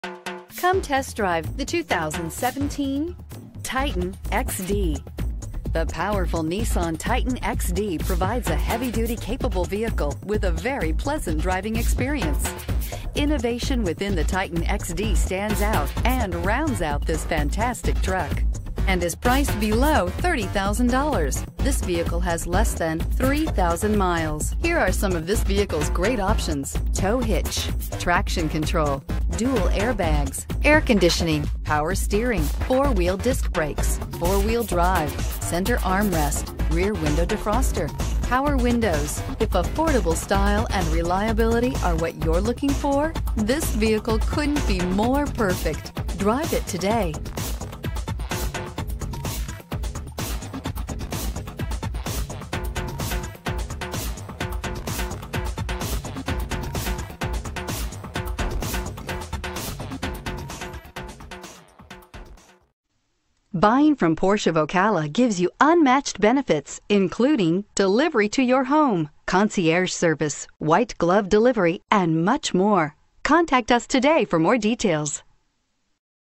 2017 Nissan Titan XD RR2029
2017 NISSAN TITAN XD Ocala, FL
Stock# RR2029

Porsche of Ocala
3949 SW College Rd

Certified Vehicle! New Arrival! CarFax 1-Owner, LOW MILES, Value Priced below the market average! -4X4 4WD -Bluetooth -Aux. Audio Input ABS Brakes -AM/FM Radio and many other amenities that are sure to please. Based on the excellent condition of this vehicle, along with the options and color, this Nissan Titan XD is sure to sell fast. -CARFAX 1-Owner